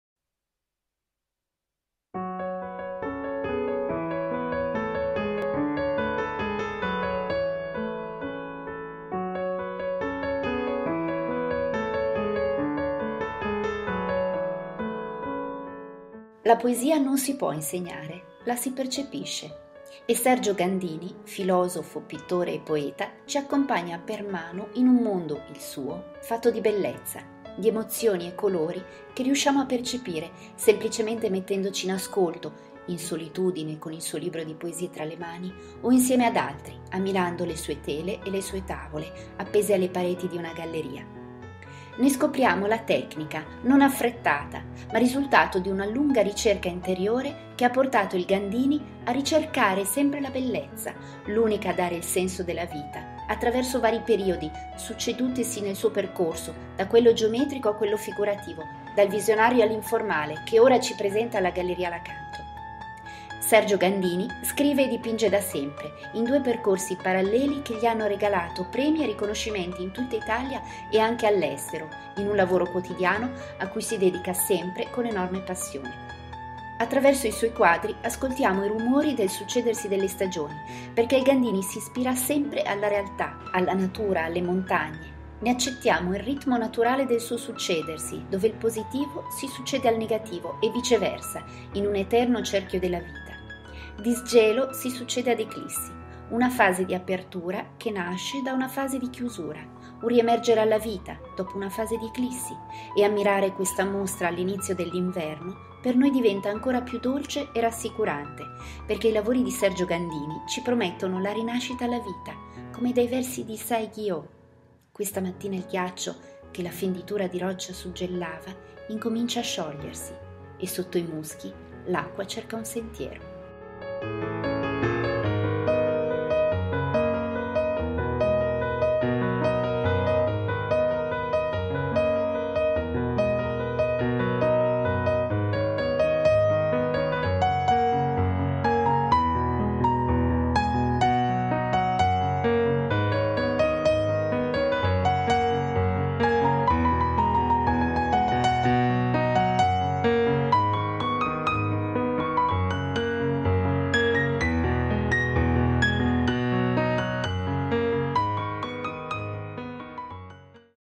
DISGELO Personale di Sergio Gandini
Con presentazione di Ester Mistò.

"DISGELO"
Personale di SERGIO GANDINI
A cura di Ester Mistò
Vernissage: giovedì 3 Novembre 2011, ore 18
Finissage: giovedì 24 Novembre 2011, ore 18

La poesia non si può insegnare: la si percepisce. E Sergio Gandini -- filosofo, pittore e poeta -- ci accompagna per mano in un mondo -- il suo -- fatto di bellezza, di emozioni e colori che riusciamo a percepire, semplicemente mettendoci in ascolto, in solitudine, con il suo libro di poesie tra le mani, o insieme ad altri, ammirando le sue tele e le sue tavole, appese alle pareti di una galleria.
Ne scopriamo la tecnica, non affrettata, ma risultato di una lunga ricerca interiore che ha portato il Gandini a ricercare sempre la bellezza -- l'unica a dare il senso alla vita, attraversando vari periodi, succedutisi nel suo percorso, da quello geometrico a quello figurativo, dal visionario e all'informale che ora ci presenta alla Galleria L'Acanto.
Sergio Gandini scrive e dipinge da sempre, in due percorsi paralleli che gli hanno regalato premi e riconoscimenti in tutta Italia e anche all'estero, in un lavoro quotidiano a cui si dedica sempre con enorme passione. Attraverso i suoi quadri, ascoltiamo i rumori del succedersi delle stagioni -- perché il Gandini si ispira sempre alla realtà, alla natura, alle montagne -- ne accettiamo il ritmo naturale del suo succedersi, dove il positivo si succede al negativo e viceversa, in un eterno cerchio della vita.
"Disgelo" si succede ad "Eclissi": una fase di apertura che nasce da una fase di chiusura, un riemergere alla vita, dopo una fase di eclissi, e ammirare questa Mostra all'inizio dell'inverno per noi diventa ancora più dolce e rassicurante, perché i lavori di Sergio Gandini ci promettono la rinascita alla vita, come da i versi di Saigyo: "Questa mattina il ghiaccio che la fenditura di roccia suggellava, incomincia a sciogliersi, e sotto i muschi, l'acqua cerca un sentiero".

Galleria L'Acanto
Via Enrico Nöe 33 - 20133 Milano